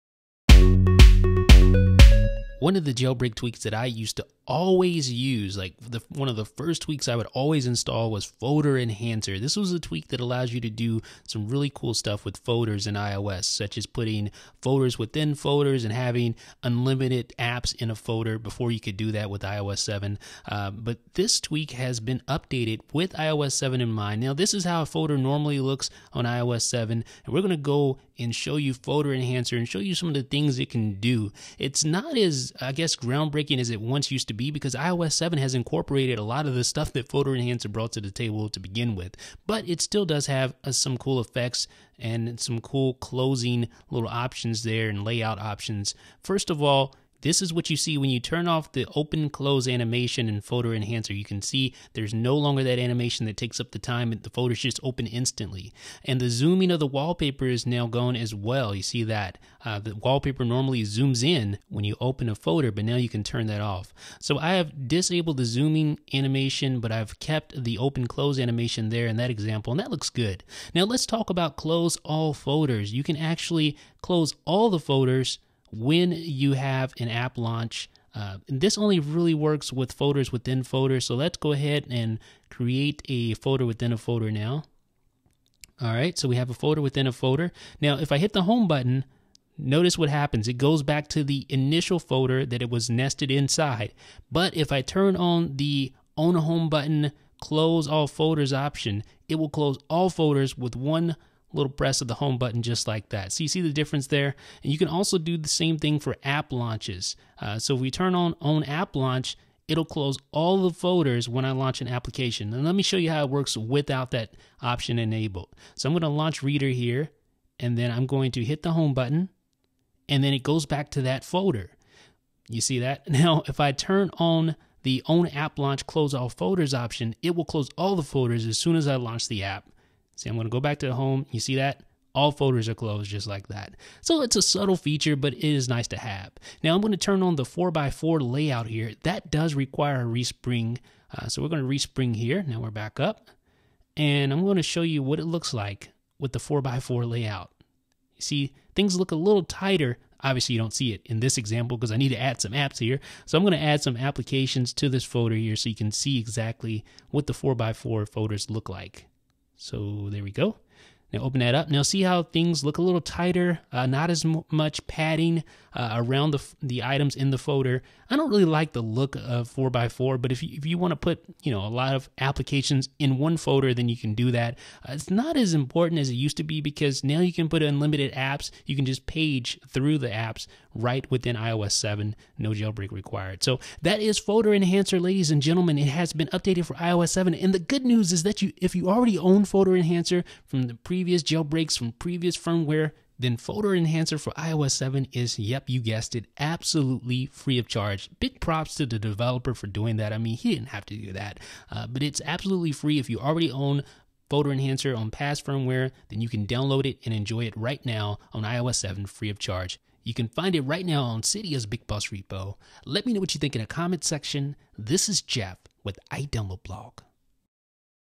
FolderEnhancer: customize folders in iOS 7
Description: FolderEnhancer is a recently updated jailbreak tweak for iOS 7. It allows you to change folder animations, and add 4x4 icon support, and add folders within folders.

Developer: Lance Fetters
Price: $0.99
Repo: BigBoss

About iDB